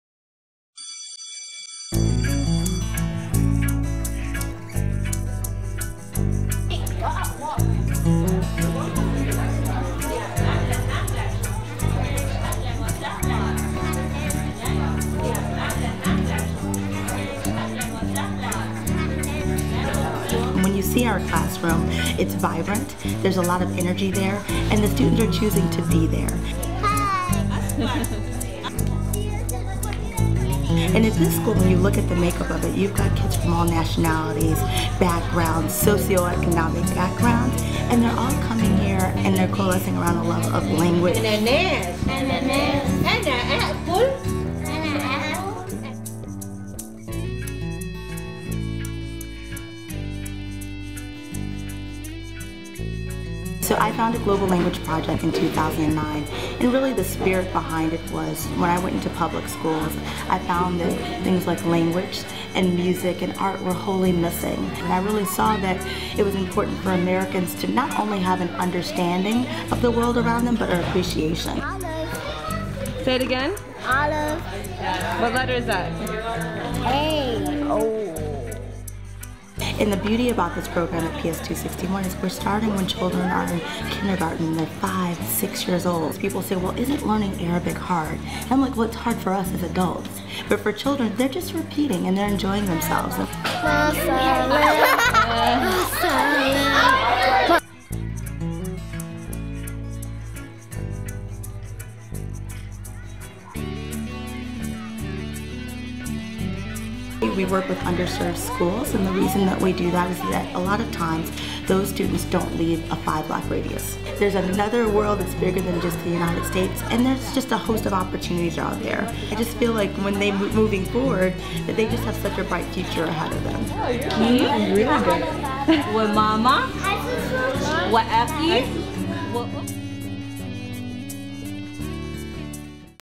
Qatar Foundation International Sponsors Global Language Project
Qatar Foundation International helps provide free foreign language studies to America's youth by sponsoring the Global Language Project.